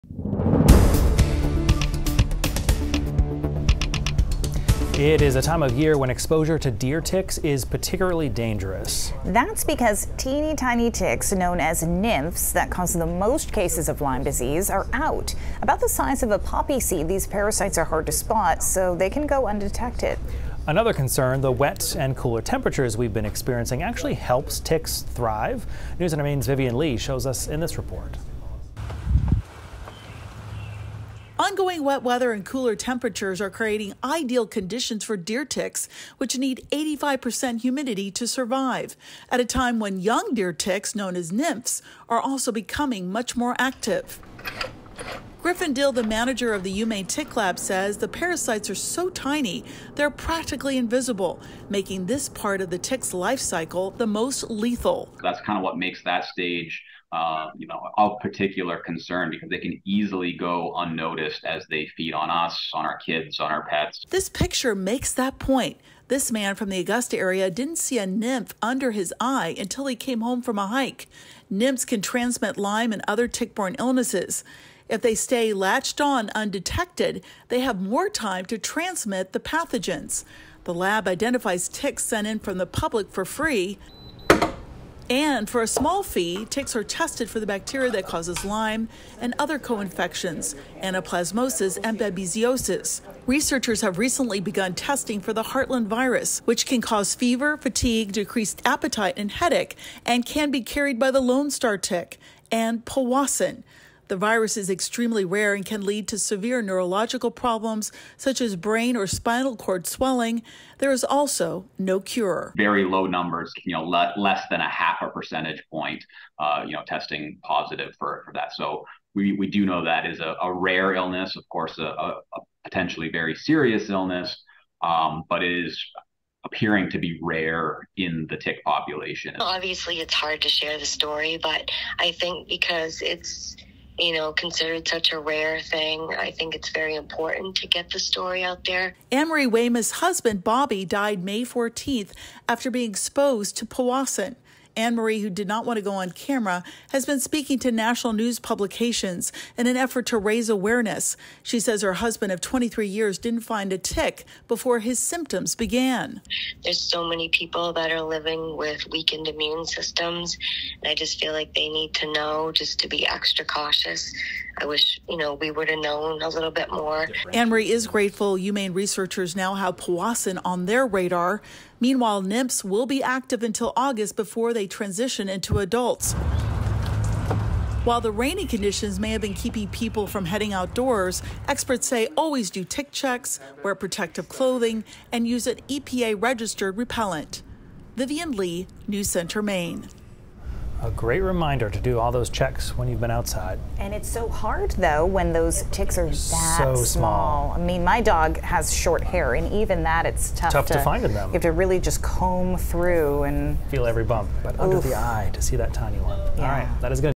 Tiny ticks that may carry tickborne diseases are out for the summer
Wet and cooler temperatures this summer are actually helping ticks thrive.